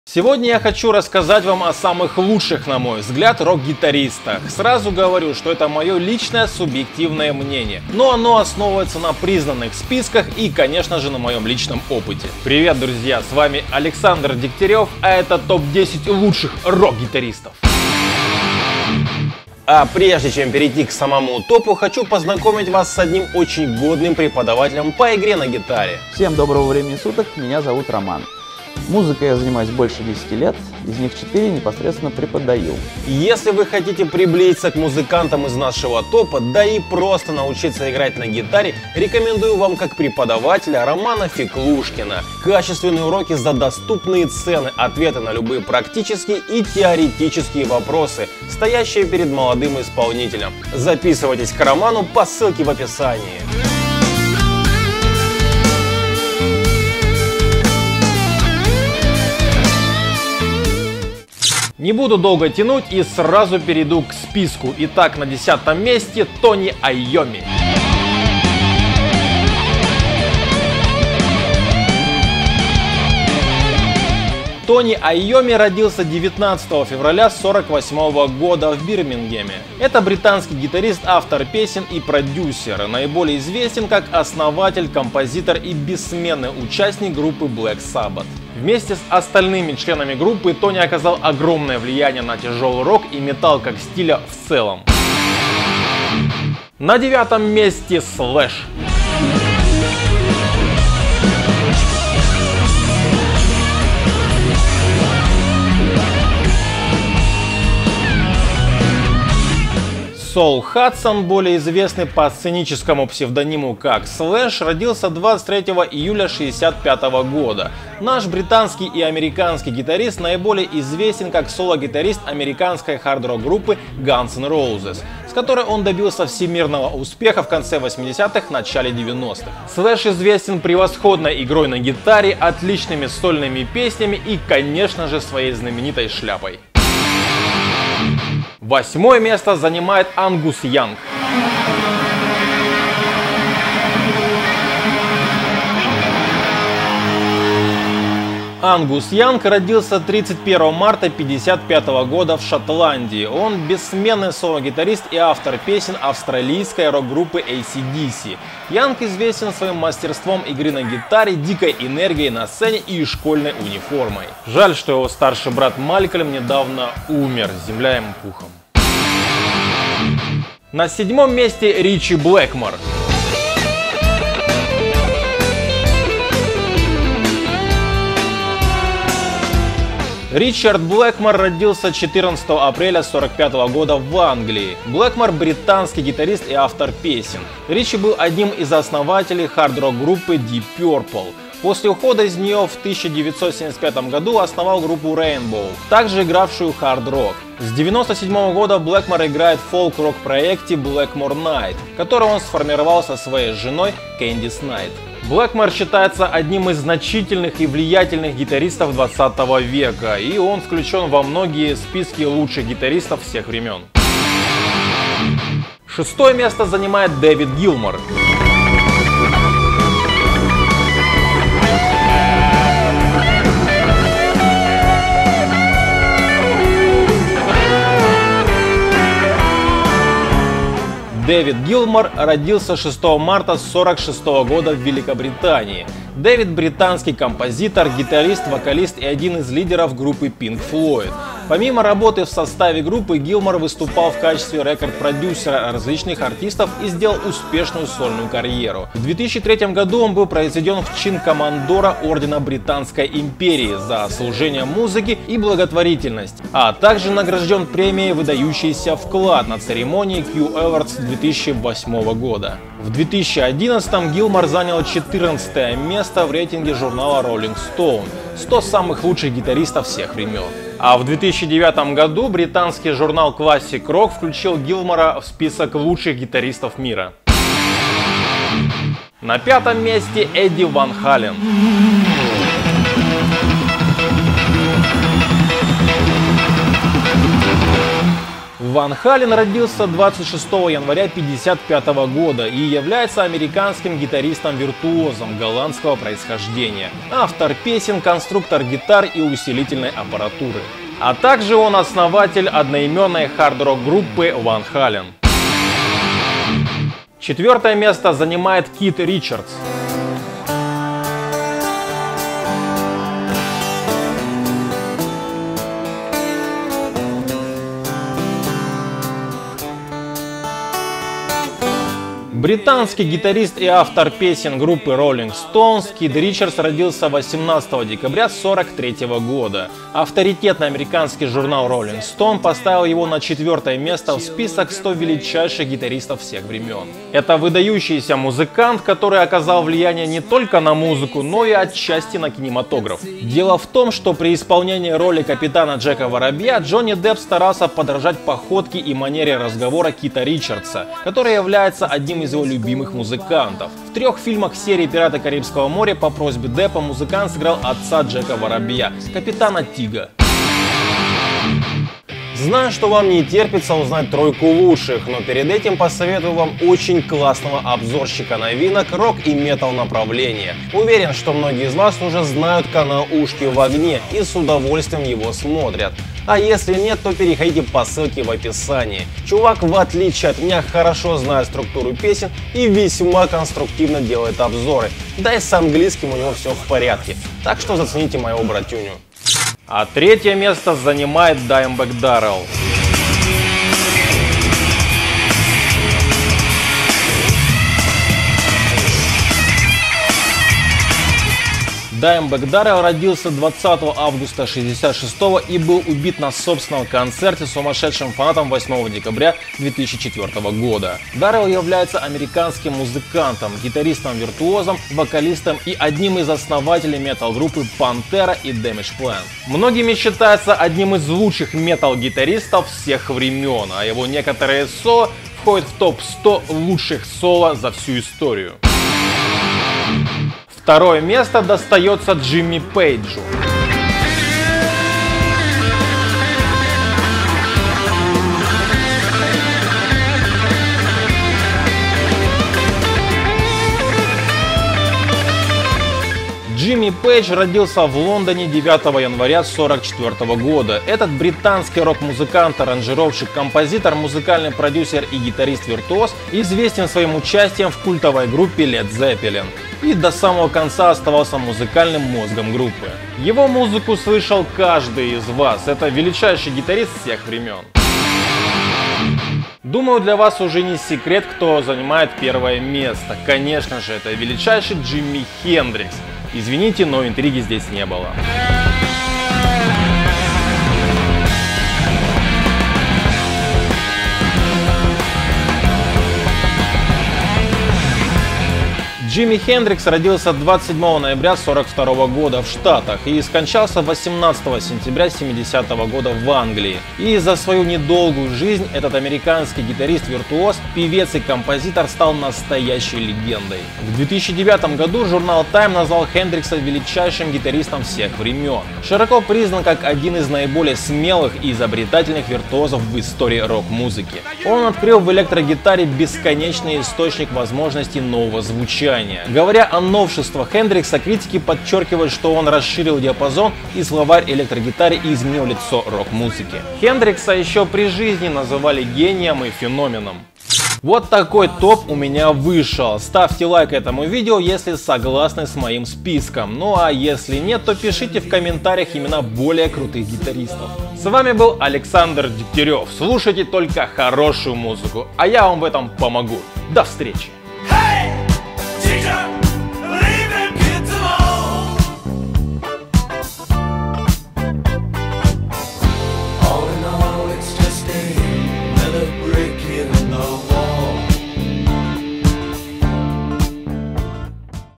ТОП 10 лучших РОК гитаристов в МИРЕ
В этом видео я хочу рассказать вам о самых лучших, на мой взгляд, рок гитаристах. Сразу говорю, что это моё личное субъективное мнение, но оно основывается на признанных списках и, конечно же, на моём личном опыте.